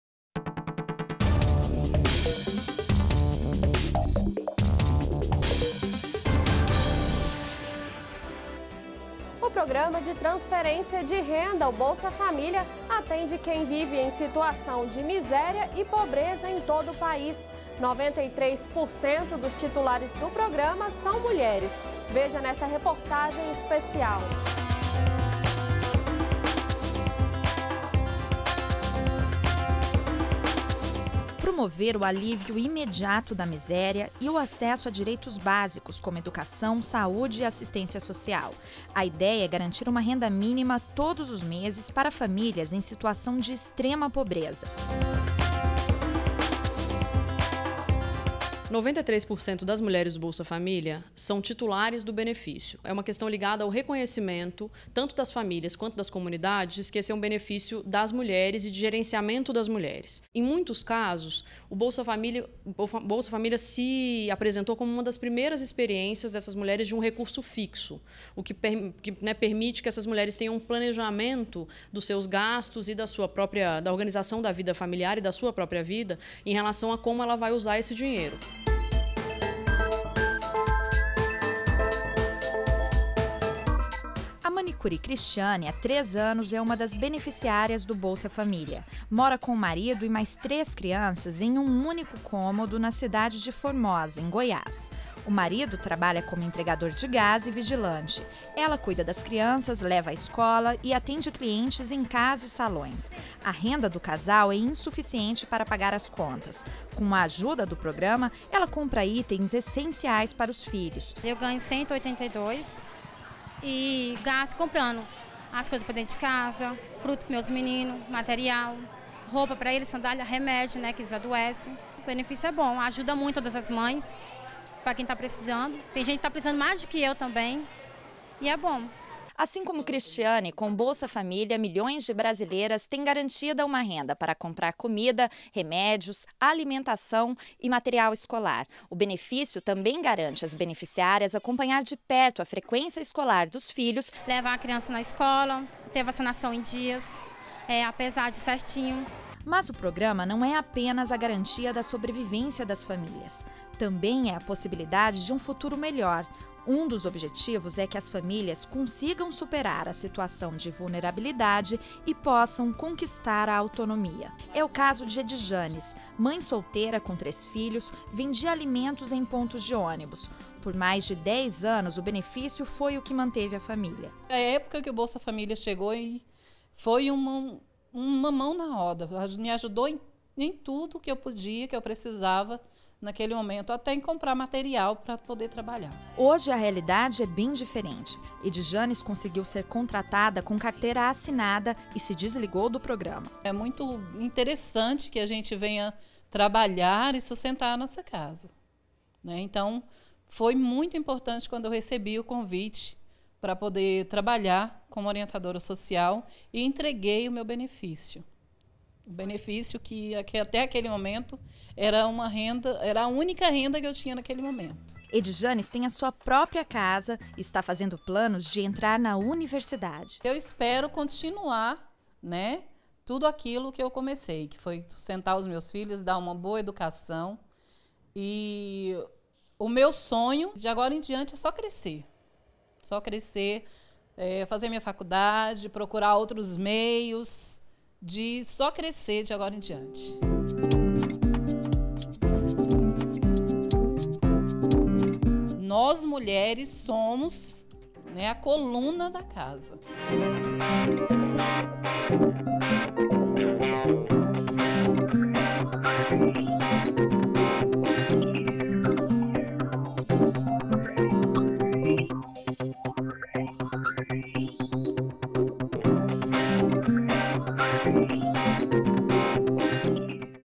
Cerca de 93% dos titulares do Programa Bolsa Família são mulheres
DA REDAÇÃO - 20.03.15: O Bolsa Família atende quem vive em situação de miséria e pobreza em todo o país. Cerca de 93% dos titulares do programa são mulheres. Alguns dos objetivos são promover o alívio imediato da miséria e o acesso aos direitos básicos como: educação, saúde e assistência social, além de incentivar os beneficiários a superar a situação de vulnerabilidade para que eles possam conquistar autonomia.